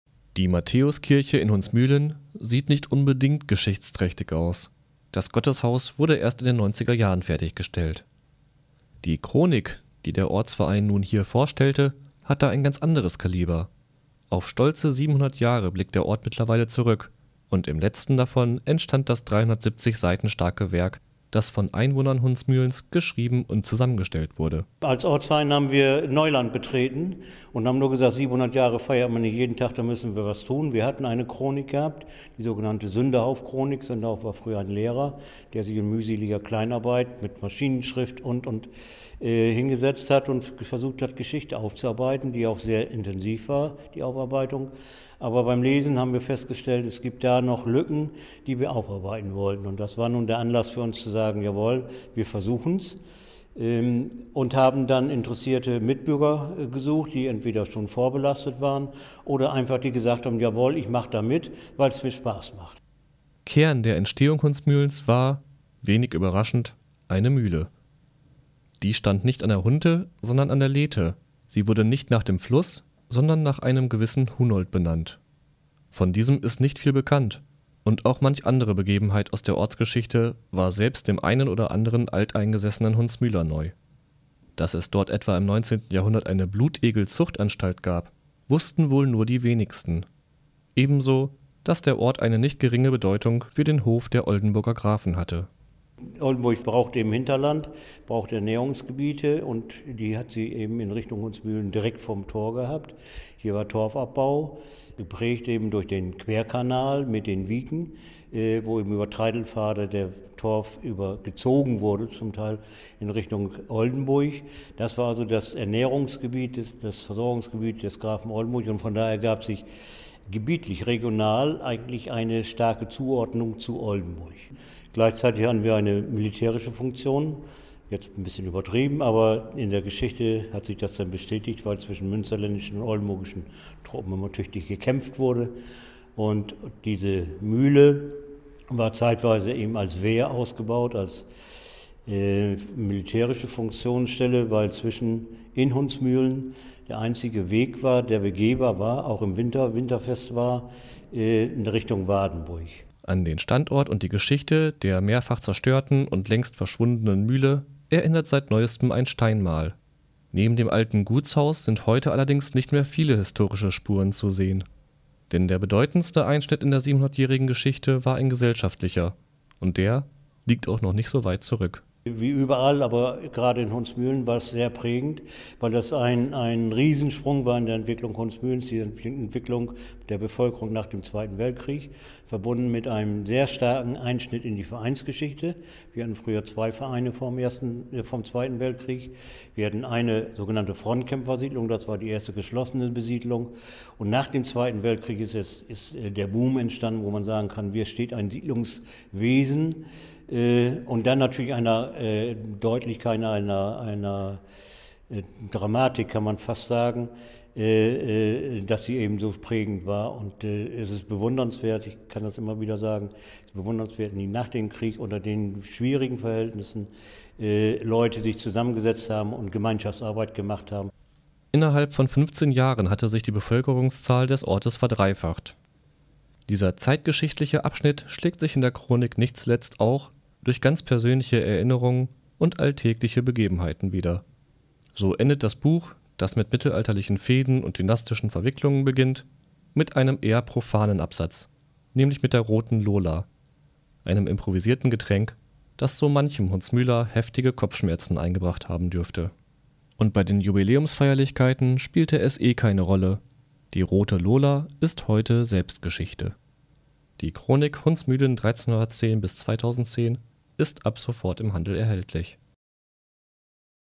700 Jahre Hundsmühlen
Hundsmühlen (Gemeinde Wardenburg) feiert in diesem Jahr den 700. Geburtstag. Zu diesem Zweck haben die Bürger nun eine Ortschronik vorgelegt.